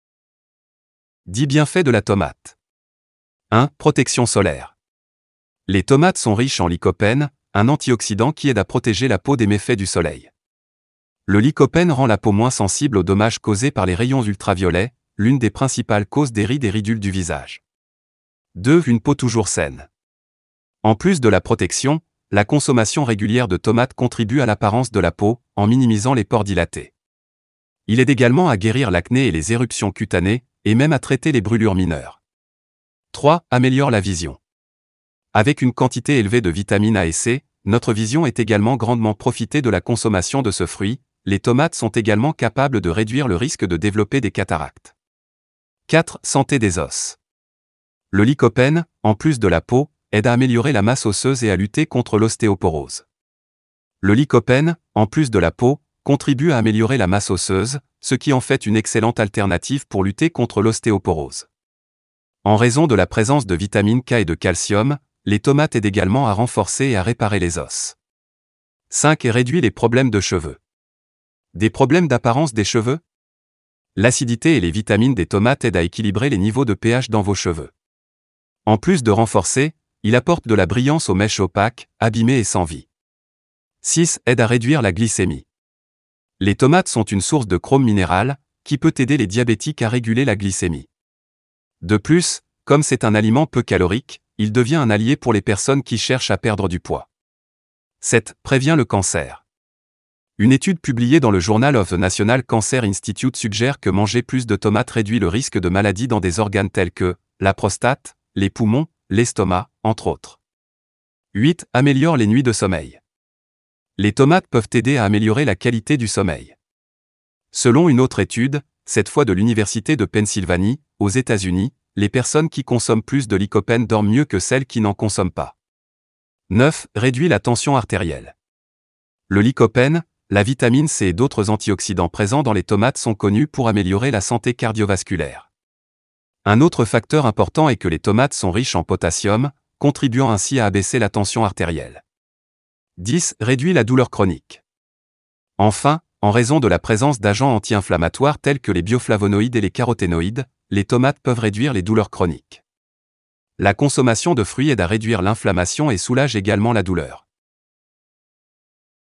10 bienfaits de la tomate
10 bienfaits de la tomate.les tomates sont riches en lycopène, un antioxydant qui aide à protéger la peau des méfaits du soleil. « Le lycopène rend la peau moins sensible aux dommages causés par les rayons ultraviolets, l'une des principales causes des rides et ridules du visage..